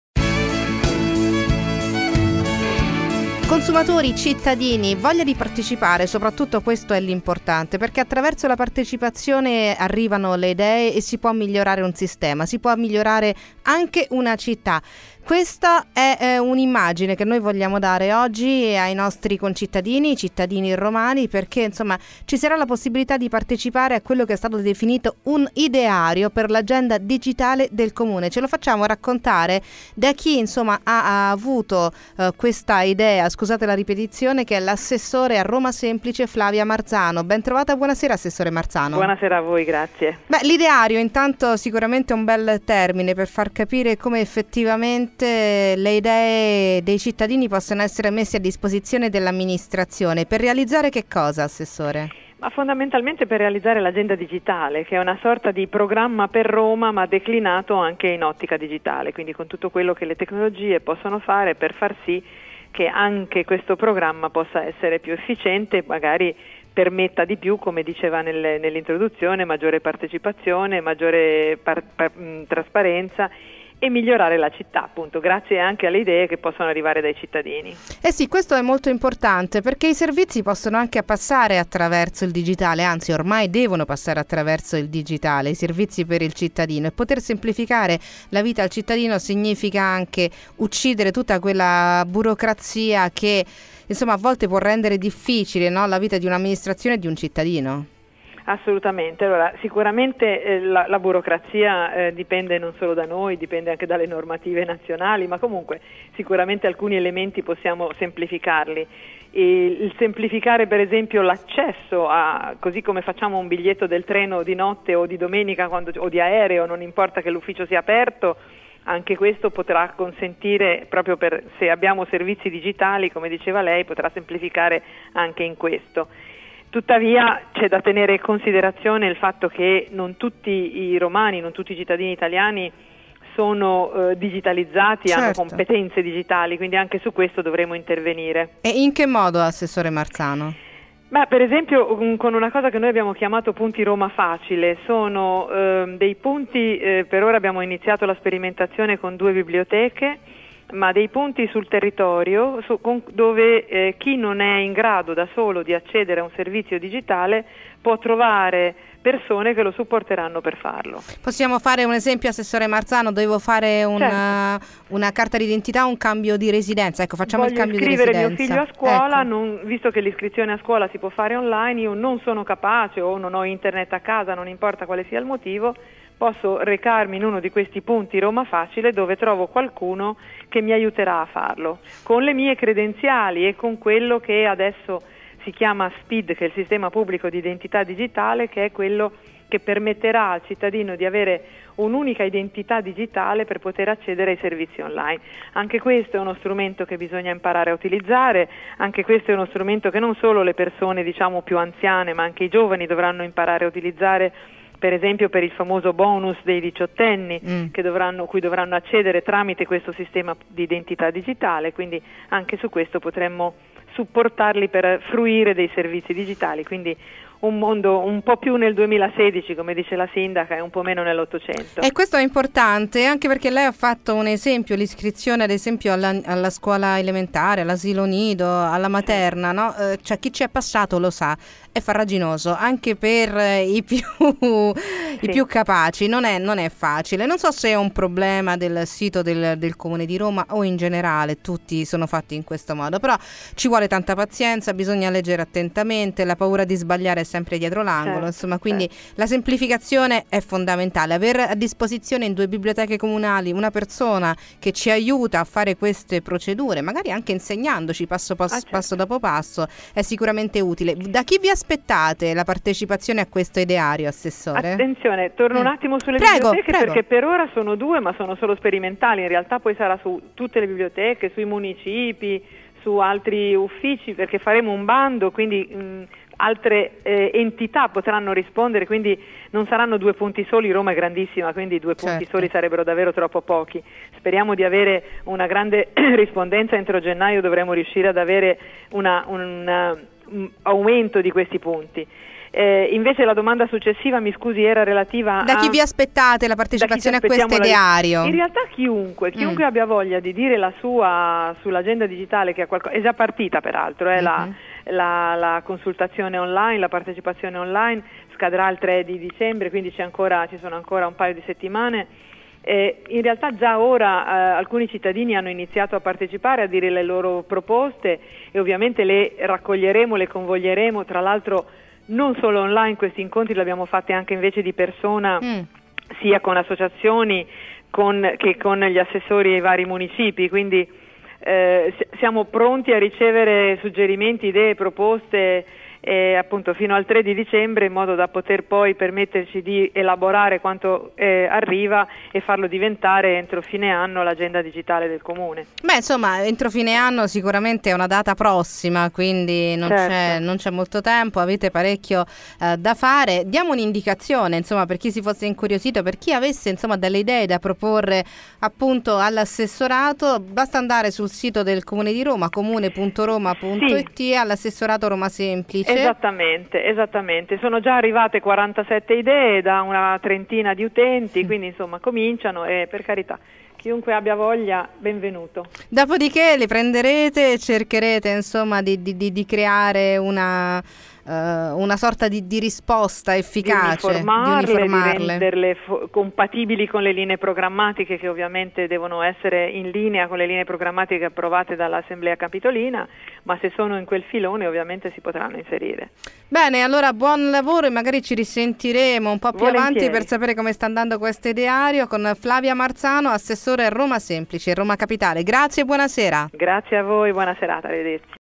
Flavia Marzano a Radio Cusano Campus - 21/11/2016
L' Assessora parla di Agenda digitale, partecipazione e inclusione dei cittadini.